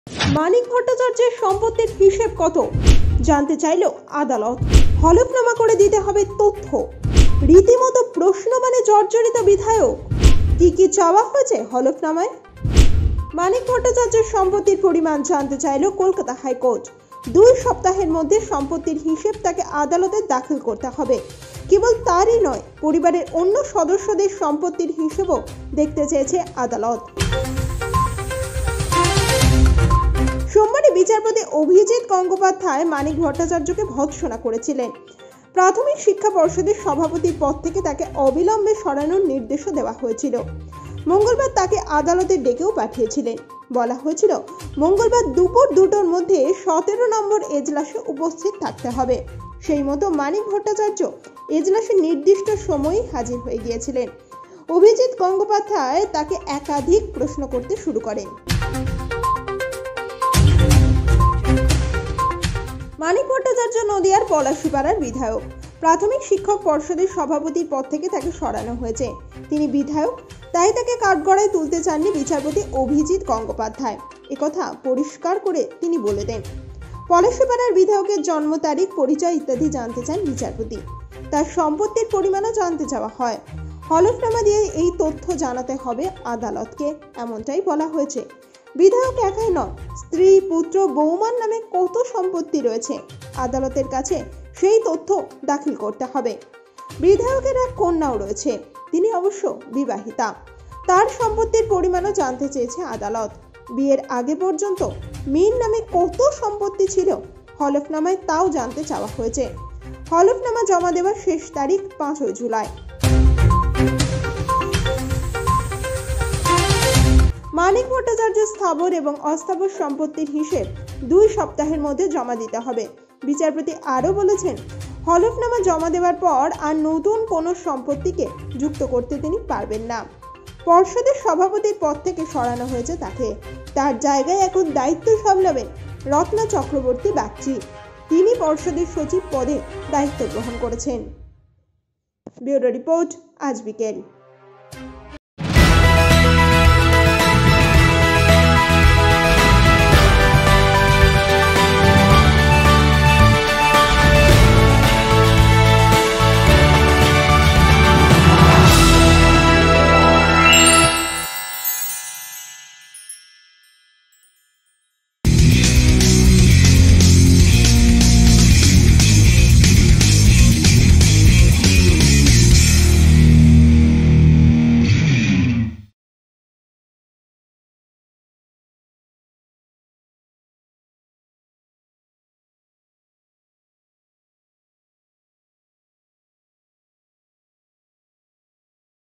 মানিকের সম্পত্তি কত? জানতে চাইল আদালত। HC seeks details of Manik Bhattacharya’s property details
The Calcutta High Court on Tuesday wanted to know details of Manik Bhattacharya’s property. He has been instructed to submit property details to the court within the next two weeks. Not only his property asset details, but property details of his family members have to be submitted too. Justice Abhijit Gangopadhyay of the Calcutta High Court asked Manik Bhattacharya to step down from his position as the chairman of the Board of Primary Education on Monday, June 20 in connection with his alleged involvement in the Primary School Teachers recruitment case. As per reports, Secretary of the Board of Directors Ratna Chakraborty will temporarily fill in his position. Manik Bhattacharya was summoned to the court on Tuesday, June 21.